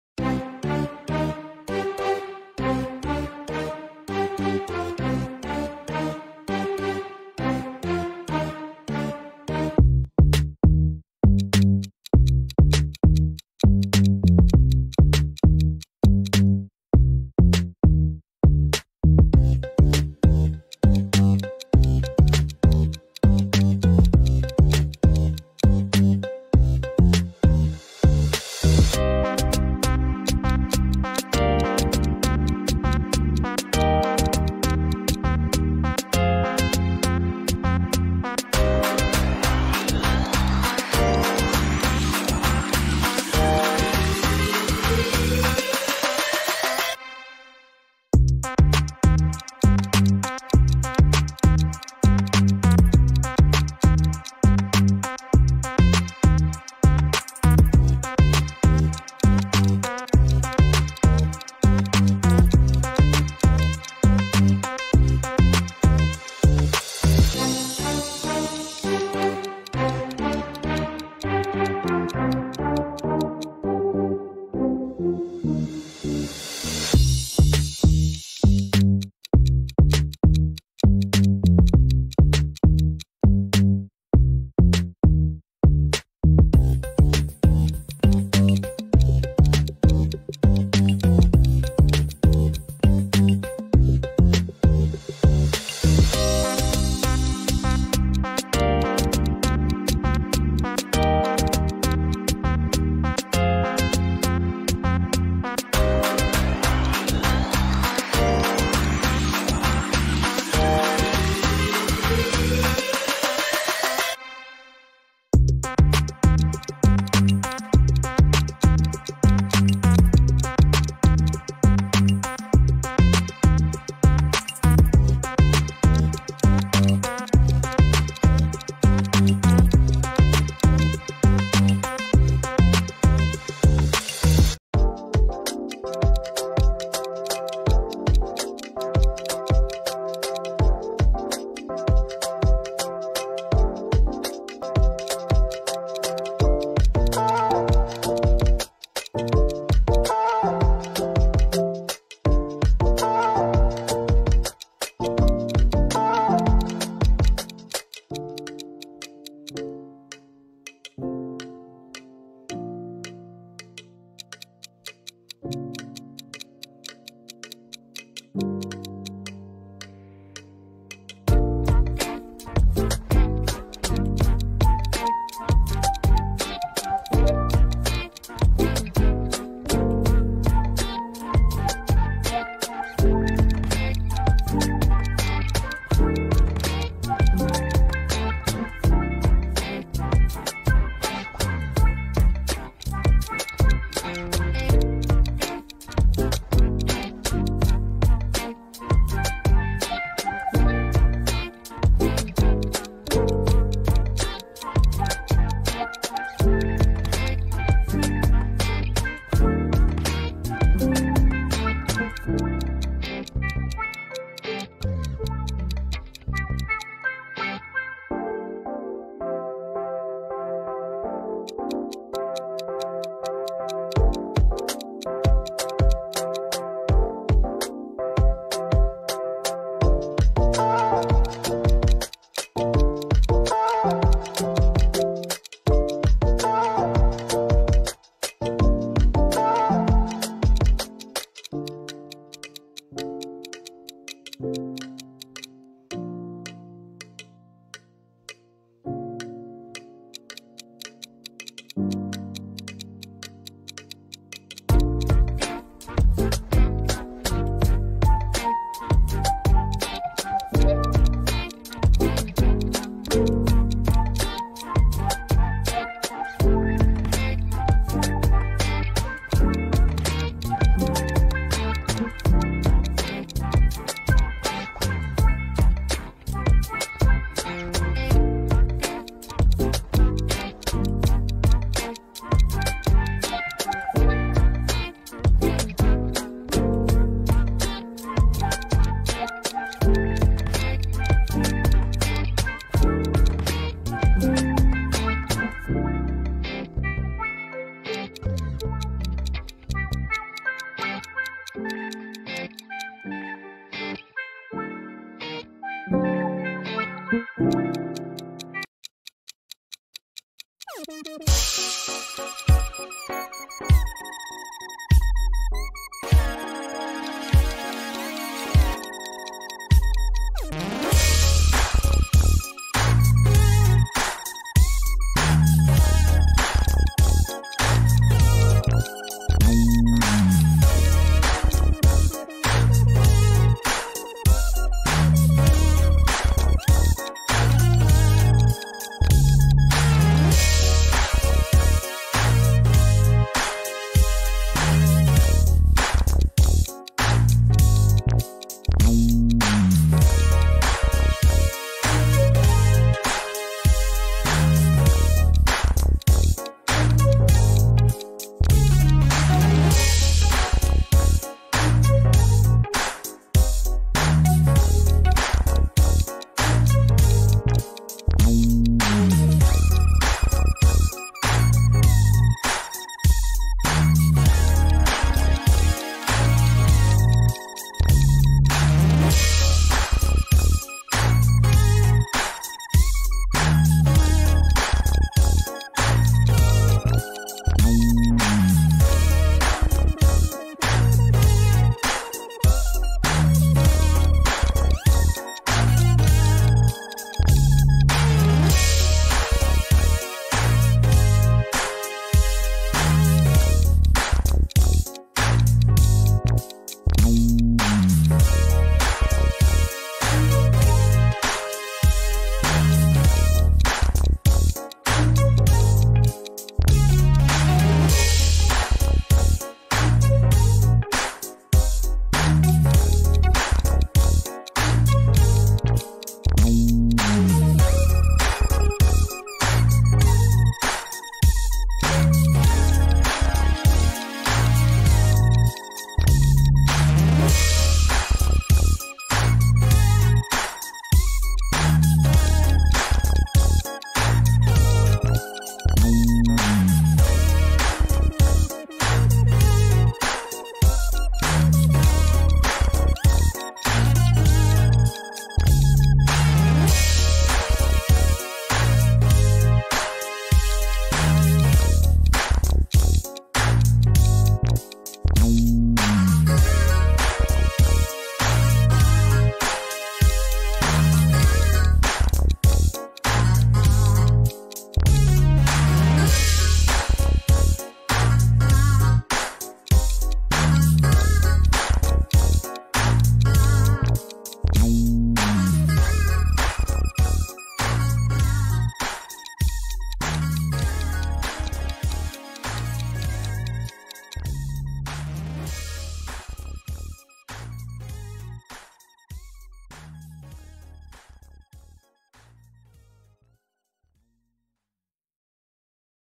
Guess the Word by Emoji Challenge! | Fun and Tricky Emoji Quiz 😃🧩| Knowledge 32
Welcome to the 'Guess the Word by Emoji' challenge! 🎉 In this fun and tricky quiz, you'll see pairs of emojis that represent a word or phrase. Can you guess what they are? Test your emoji decoding skills and challenge your friends to see who can get the most right!

Welcome to "Informative Quiz"!